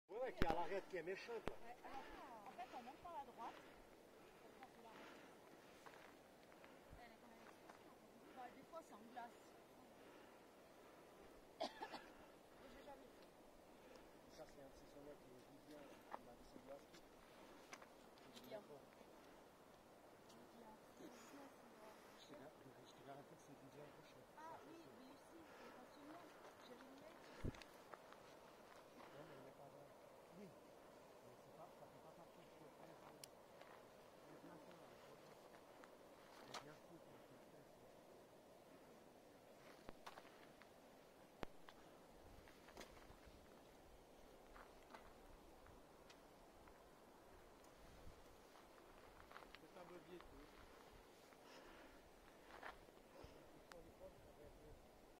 112 2/5 2023.07.11 panorama 2750m after Gässi on our way to Barrhorn
difficile physiquement mais superbe randonnée, la plus haute de Suisse, sommet à 3610m
sommet découvert dans le très utile livre de Xavier, merci à lui, sans son travail je n'aurais pas tenté plusieurs randonnées difficiles